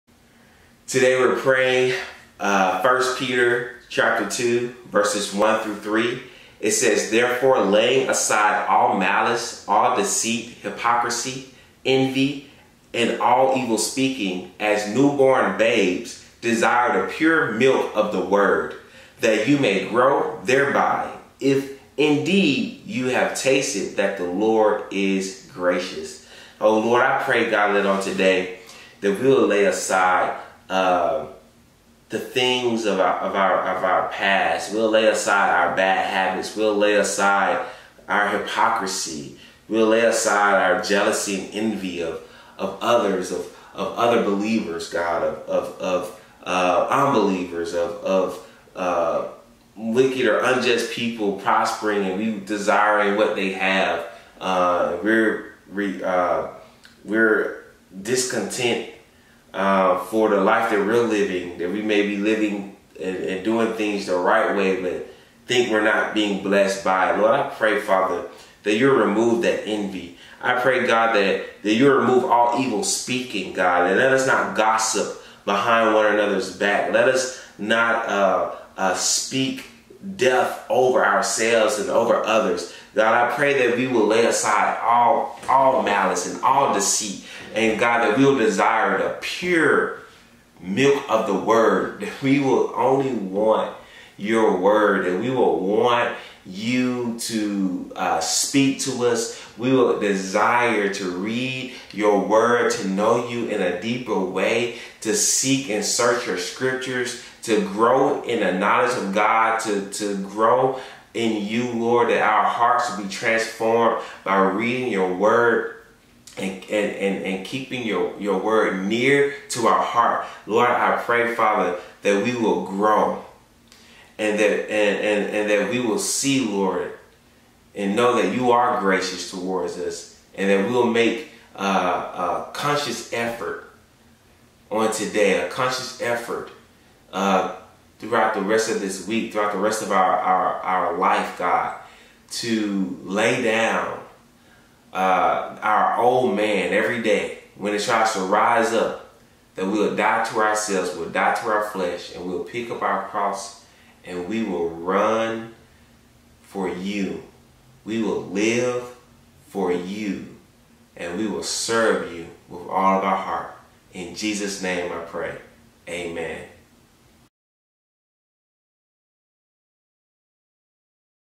Day 37 Praying 1 Peter 2:1-3
Hey Prayer Catalysts!

Join us for a 90-Day Challenge. We are modeling how to take a scripture or scriptures from the Bible and pray them over yourself or your family!

IG & Facebook: @flamesonthealtar